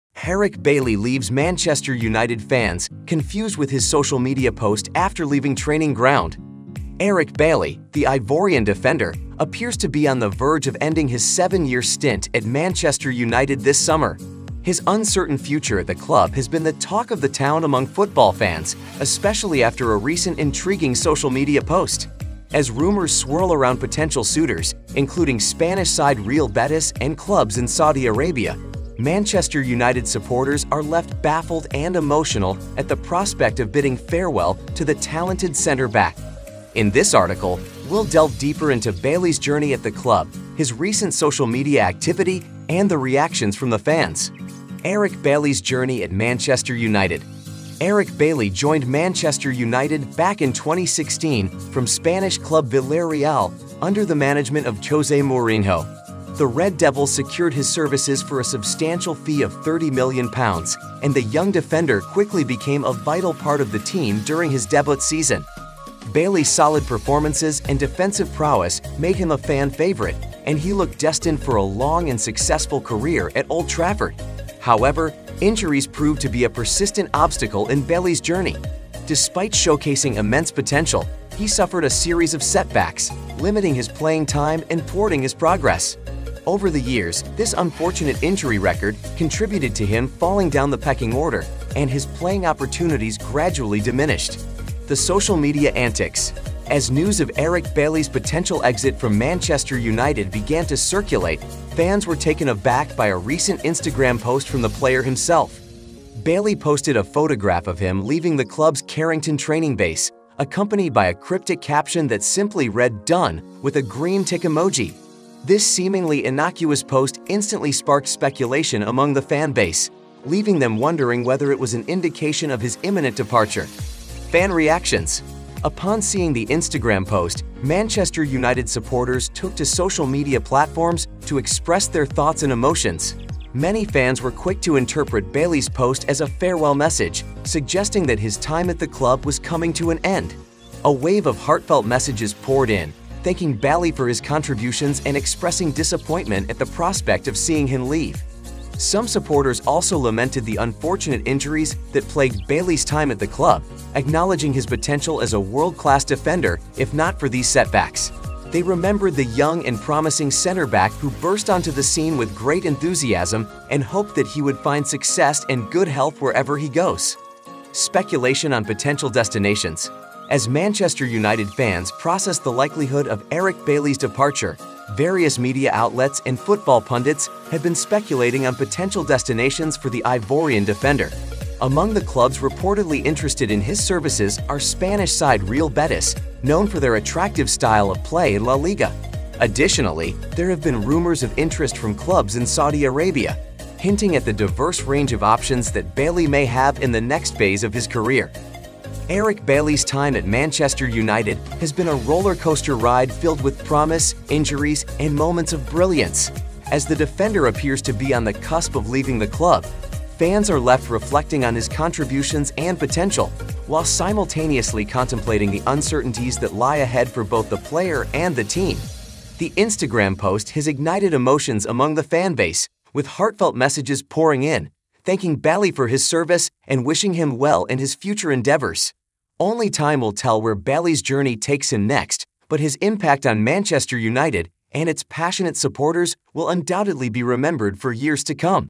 Eric Bailly Leaves Manchester United Fans Confused with His Social Media Post After Leaving Training
Title: Eric Bailly's Potential Exit from Manchester United Sparks Fan Speculation | Man Utd Transfer News

Description:
In this latest football transfer news update, we delve into the intriguing situation surrounding Eric Bailly at Manchester United. The Ivorian defender looks set to bring an end to his illustrious seven-year spell at the club this summer, leaving Red Devils fans baffled and emotional at the prospect of his departure.

Join us as we explore the recent social media antics that have set the football world abuzz, fueling speculation about Bailly's imminent move away from Old Trafford. Having fallen down the pecking order over recent seasons, the talented center-back embarked on a season-long loan deal with French side Marseille, but a potential permanent move to the Ligue 1 outfit was not pursued.

Now, with just one year remaining on his current contract, Bailly's future with Manchester United hangs in the balance. We discuss the clubs rumored to be potential suitors for his signature, with Spanish side Real Betis and several clubs in Saudi Arabia being linked to the player.

The crux of the discussion revolves around Bailly's recent Instagram post, where he bid farewell to the club's Carrington Training Base with a caption simply reading "Done" and a green tick emoji. This enigmatic post has led fans to believe that the defender is indeed on his way out of Manchester United.

Amidst the buzz surrounding Bailly's future, we also take a look at the injury setbacks that have plagued his time at the club, leaving some supporters bemoaning the potential that never fully materialized due to these setbacks. Nevertheless, his impact on the team during his debut season and moments of brilliance are remembered fondly by the fan base.

The football writer, Daniel Orme, provides insight into the complexities of Bailly's relationship with the club over the past few years and touches upon the player's accusations of an English bias when it comes to team selection.

As the transfer window heats up, we explore the potential domino effect of Bailly's departure, with the likes of Alex Telles, Axel Tuanzebe, and Phil Jones already having left the club, and former captain Harry Maguire's future also uncertain. We discuss Manchester United's defensive reinforcements during the off-season, with Jonny Evans returning to bolster the backline.

Don't miss this in-depth update on Eric Bailly's potential exit from Manchester United and the passionate reactions from fans.